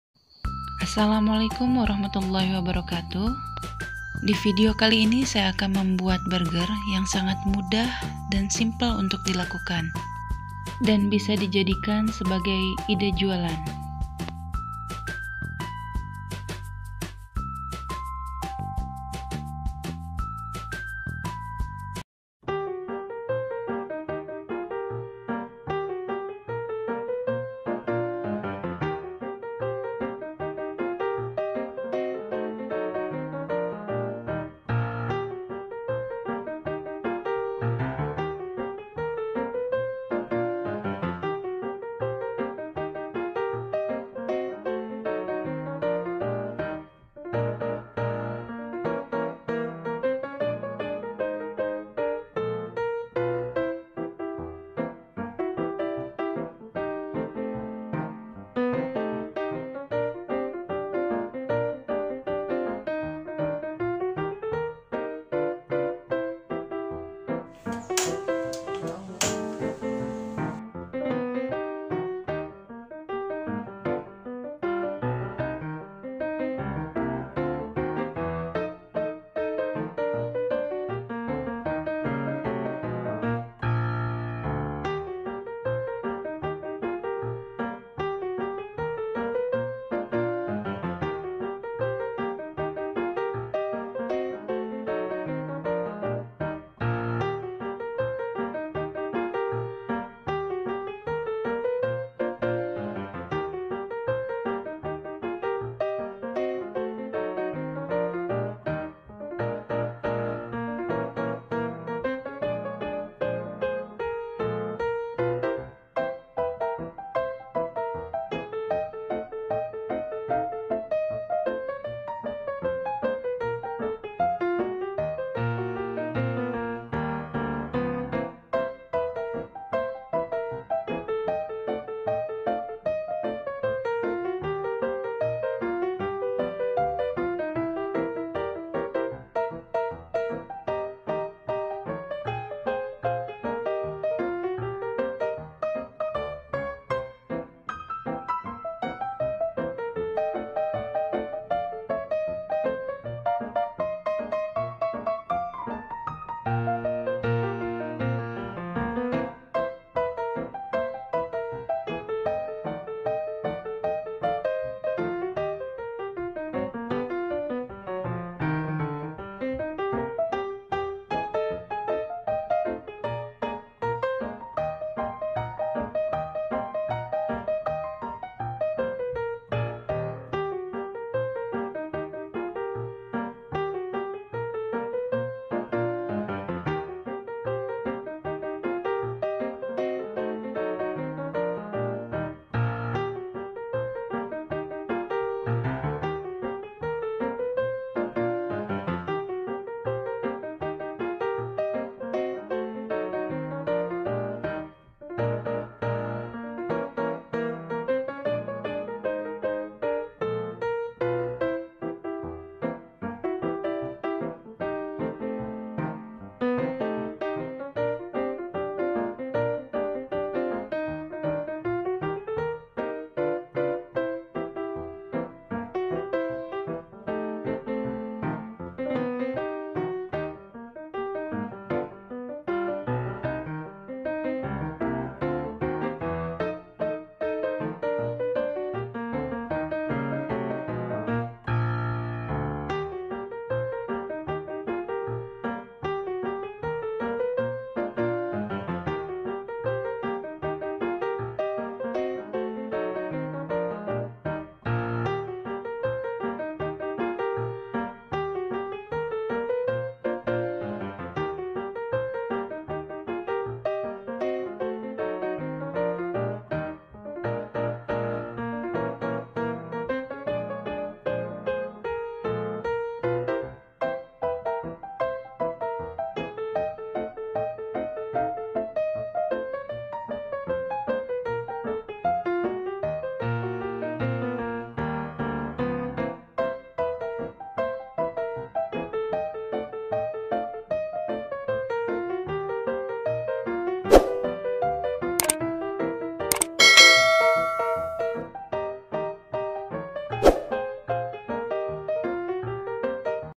Ide Jualan || Jualan burger murah di rumah
Haiii semua...
Vidio ini berisi tentang cara membuat burger Sederhana di rumah dan bisa juga sebagai ide jualan kalian .

Semoga bermanfaat
Terimakasih